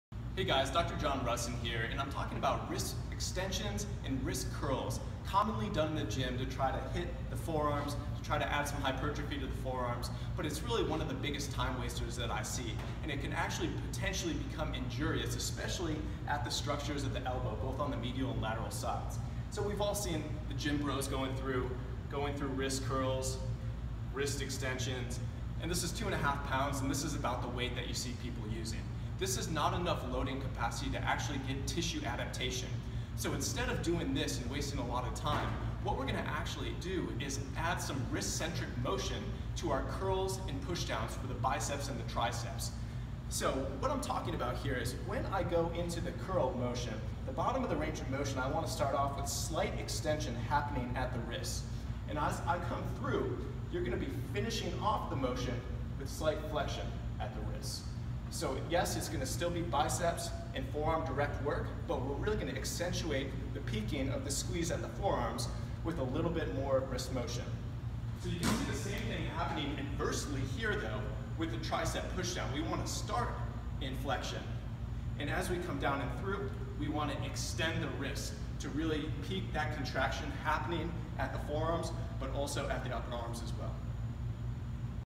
Wrist-Centric Biceps Curls & Triceps Pushdowns
In this video Dr. John Rusin dispels the direct forearm training myth and shows how to get the very most out of your biceps curls and triceps pushdowns for forearm development.